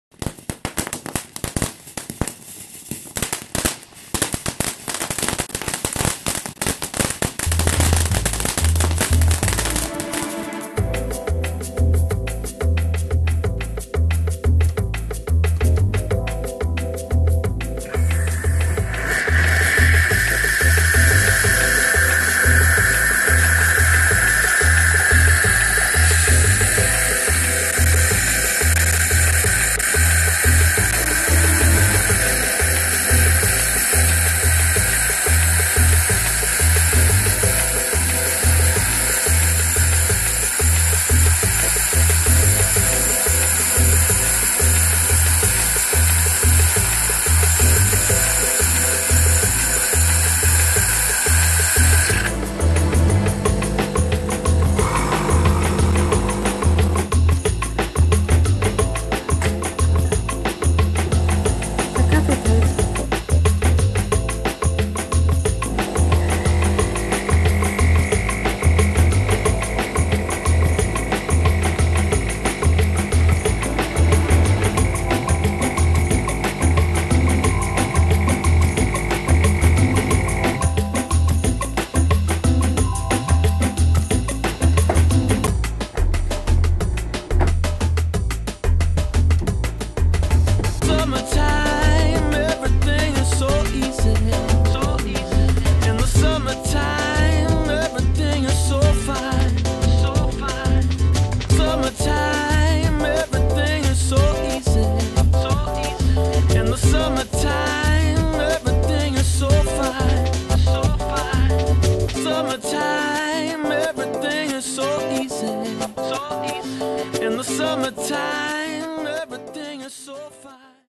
Der Kaffeegenuss von Melitta
Frisch gekochter Bella Crema Selection des Jahres Melitta Kaffee.
Ist vom Kaffeevollautomaten  einfach nur Hammer lecker.